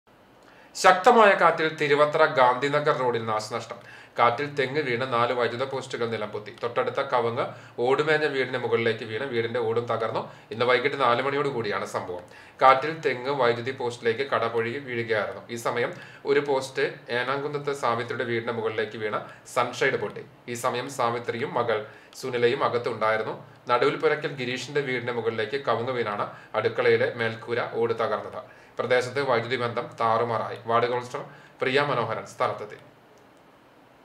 ശക്തമായ കാറ്റില്‍ തിരുവത്ര ഗാന്ധിനഗര്‍ റോഡില്‍ നാശ നഷ്ടം
കാറ്റില്‍ തെങ്ങു വീണു 4 വൈദ്യുത പോസ്റ്റ് നിലം പൊത്തി. തൊട്ടടുത്ത കവുങ്ങ് , ഓട് മേഞ്ഞ വീടിനു മുകളിലേക്ക് വീണു വീടിന്റെ ഓട് തകര്‍ന്നു. ഇന്ന് വൈകുന്നേരം 4 മണിയോടെയാണ് സംഭവം.